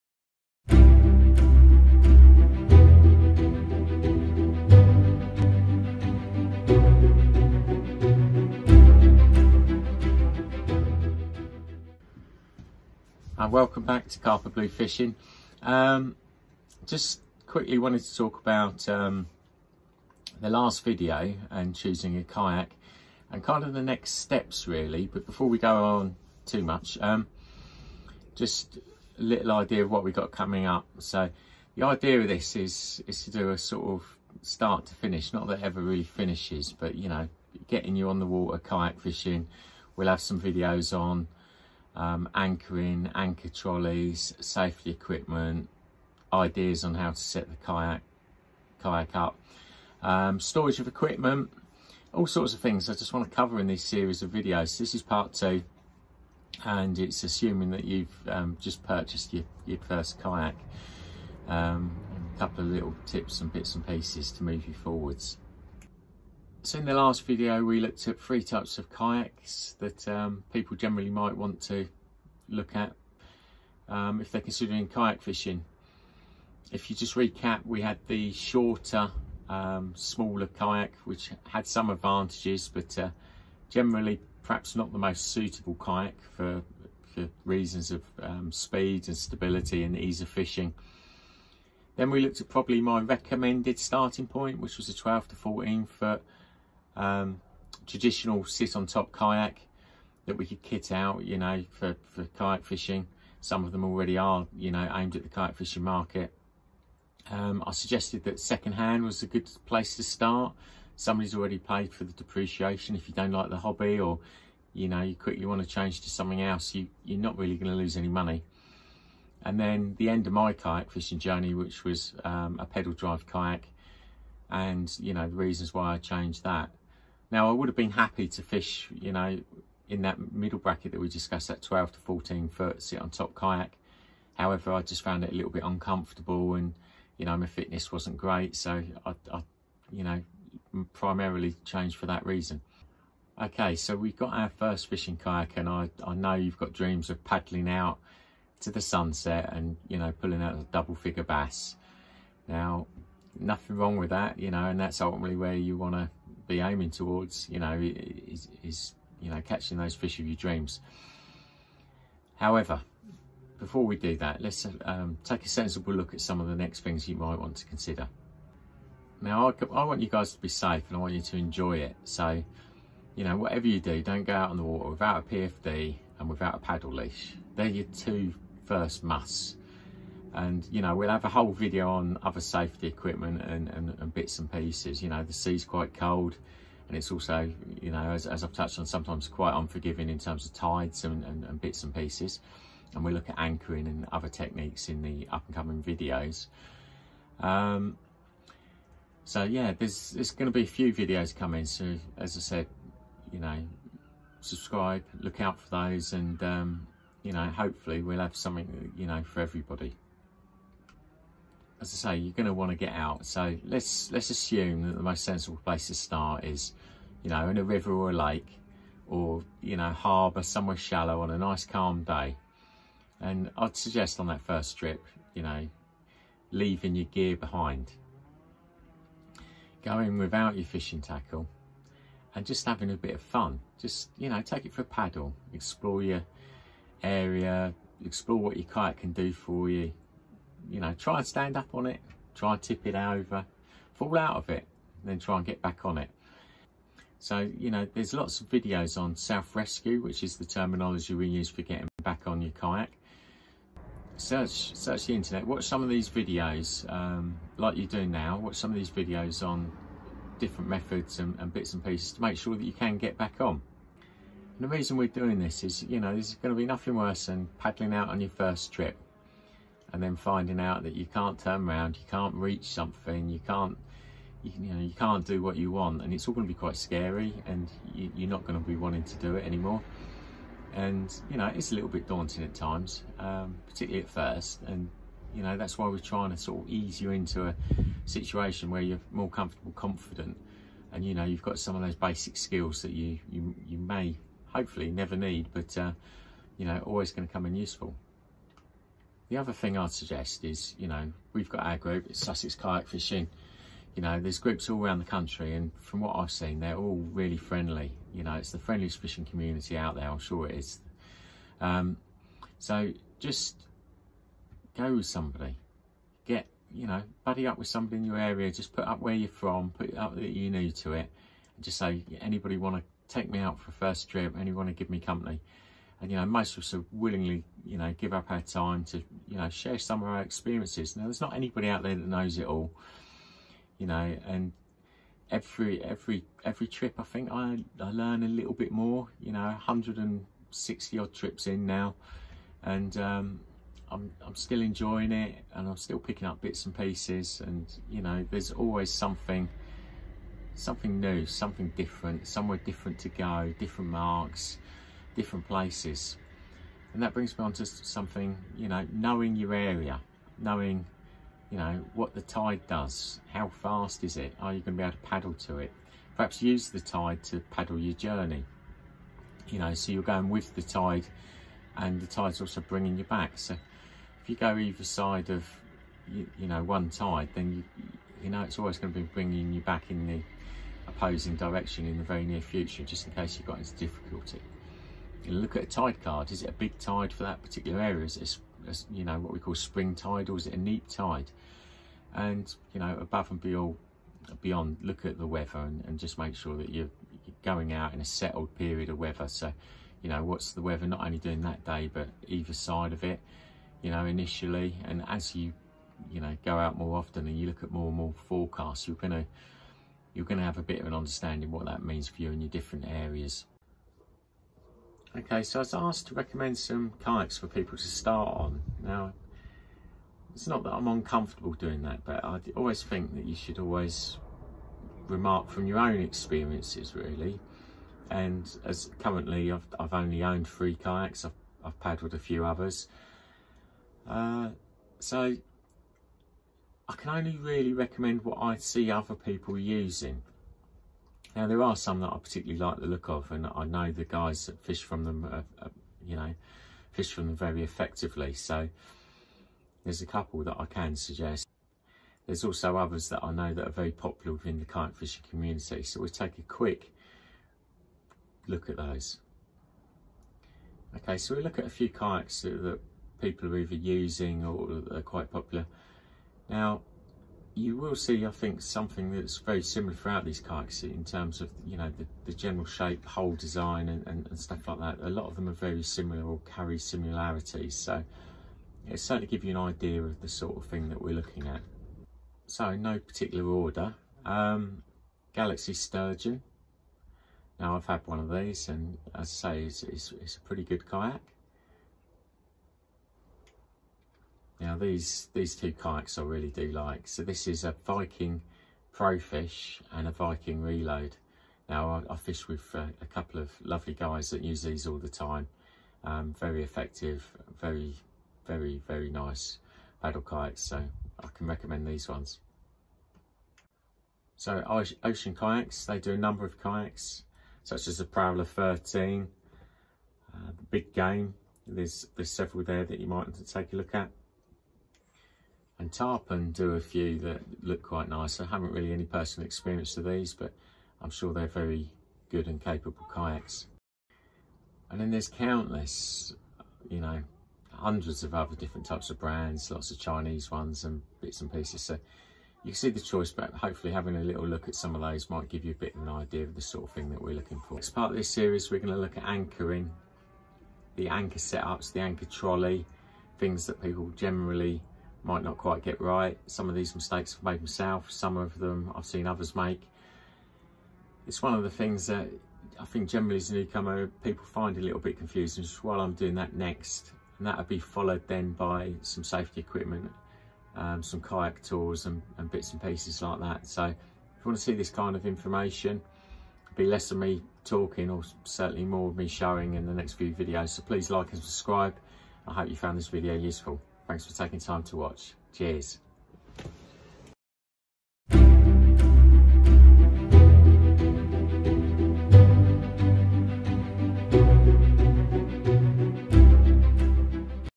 Beginners quite to Choosing a fishing kayak part 2 and those first steps. Learning To Kayak fish
The second video in a series how to choose a Fishing kayak in the UK. In this video we recap the content of the first video in the series have a brief introduction to some of the fishing kayaks available and a talk about those first steps into kayak fishing.